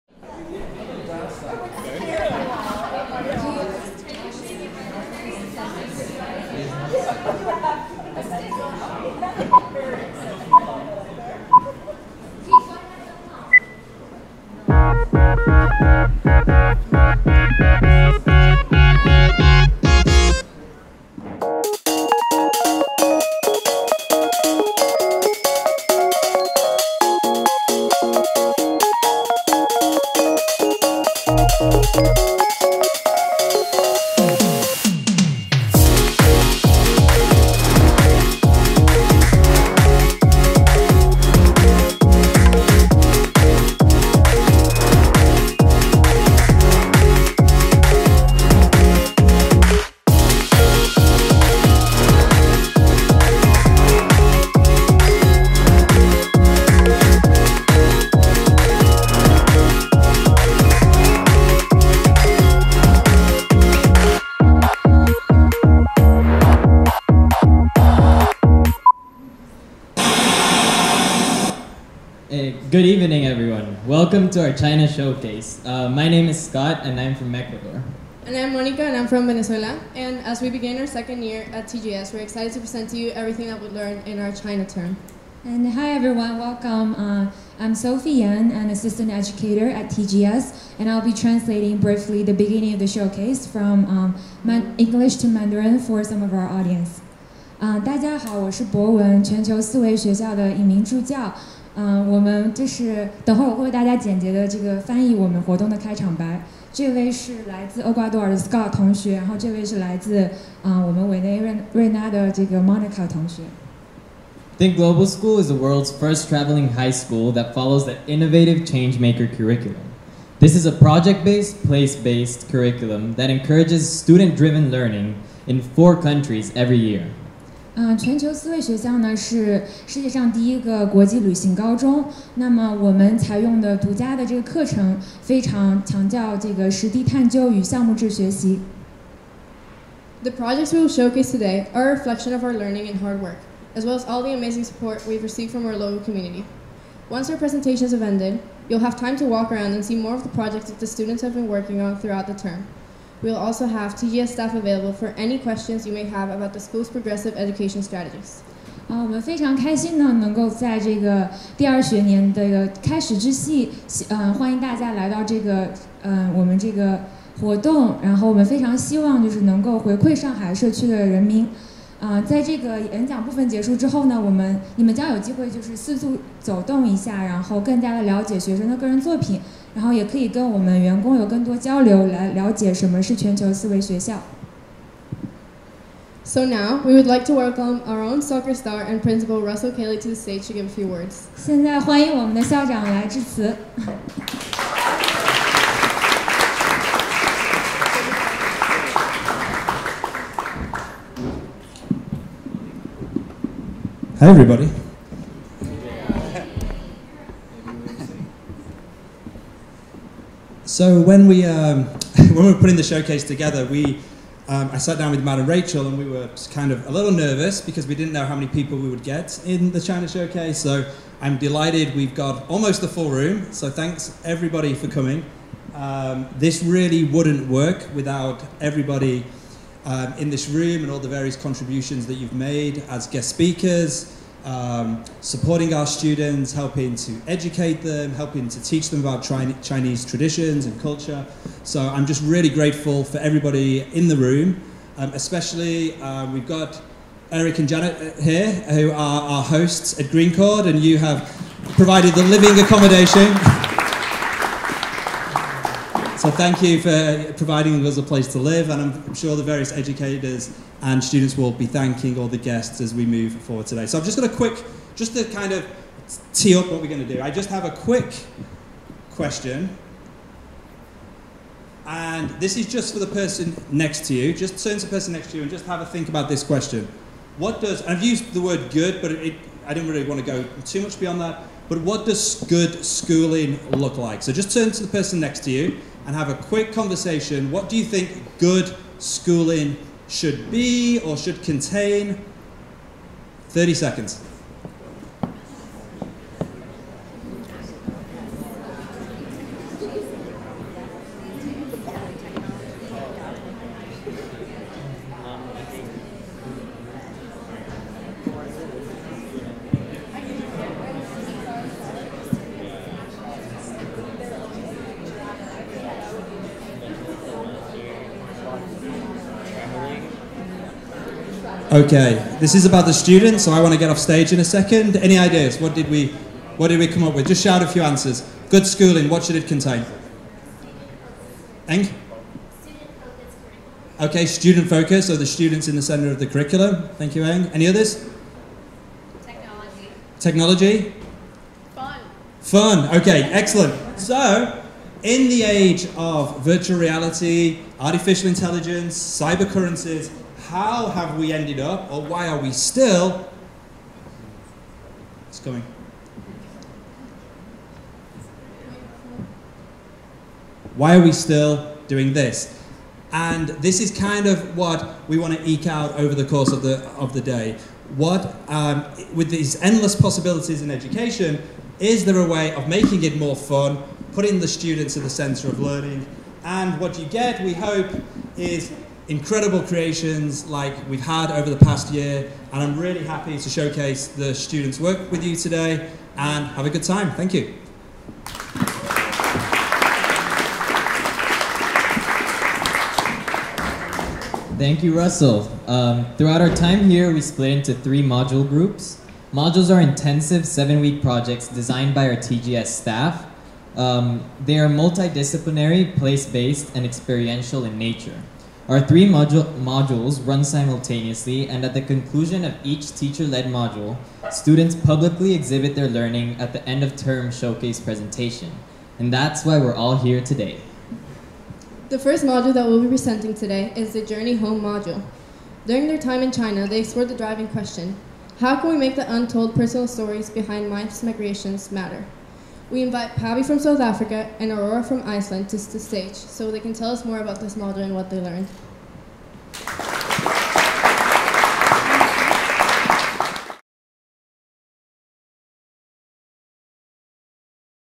1. Showcase Introduction and Principal Russell Cailey's Speech
Each term at THINK Global School ends with a project-based learning showcase, where students demonstrate what they’ve learned during the course of their personal projects and teacher-led modules.

These projects and modules typically highlight driving questions relevant to the places we live and learn, and in this showcase students discuss what  they learned during their time in China.